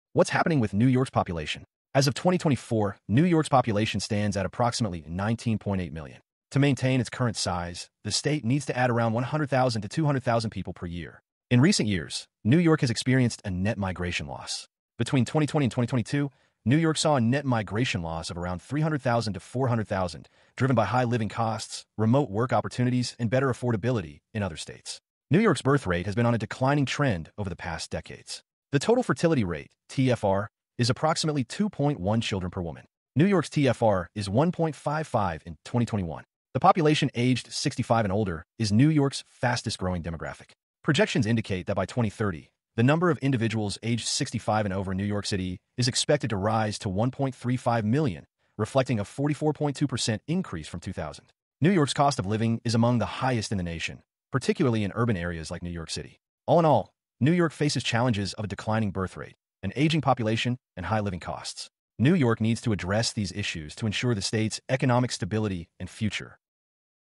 New York's Population Dilemma Trends and Challenges
New York's population is at a crossroads. With a declining birth rate, an aging population, and years of net migration loss, the state's future is uncertain. Will immigration be enough to sustain growth, or is New York headed for a demographic crisis? Watch now to uncover the trends and challenges shaping the Empire State.